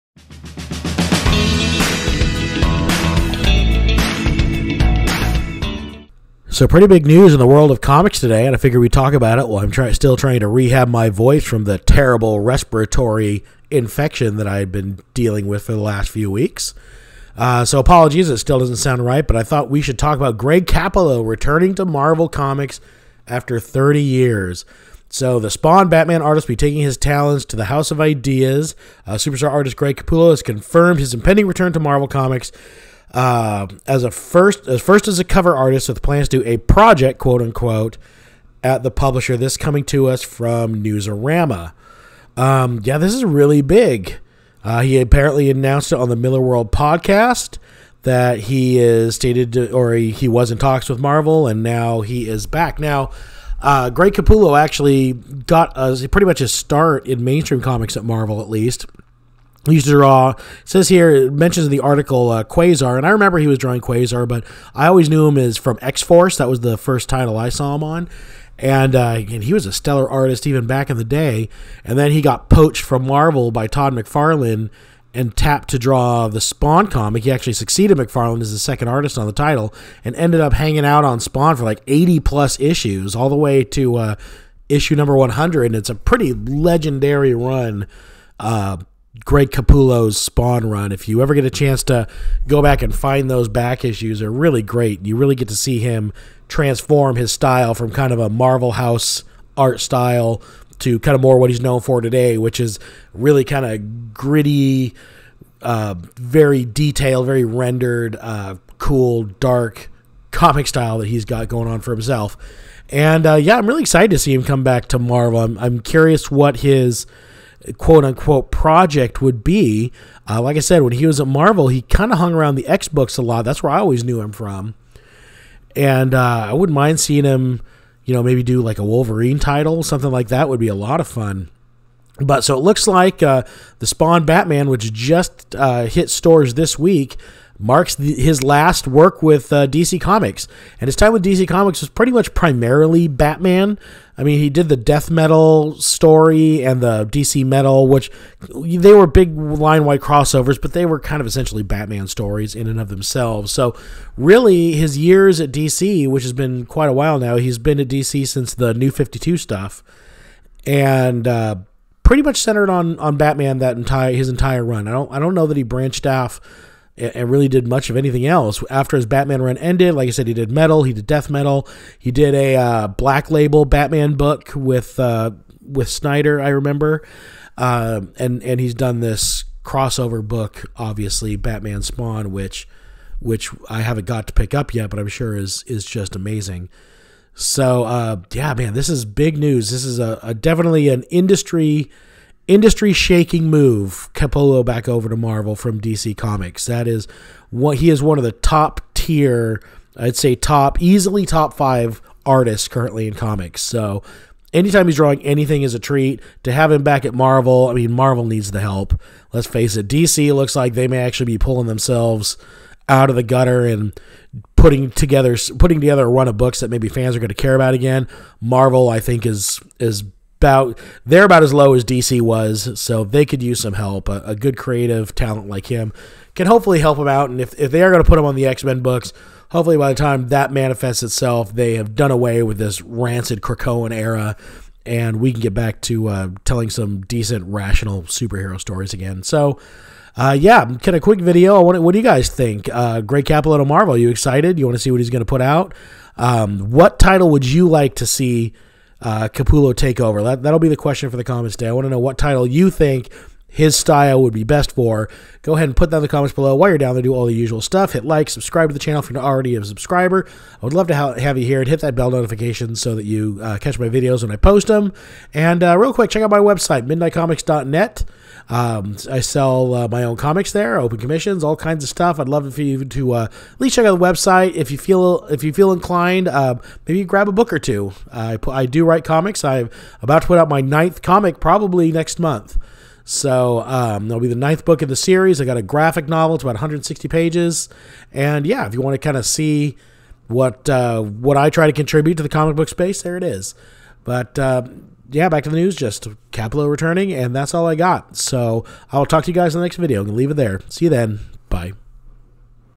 Batman Artist Greg Capullo Is Marvel Bound!
Marvel snags a top industry artist away from DC Comics!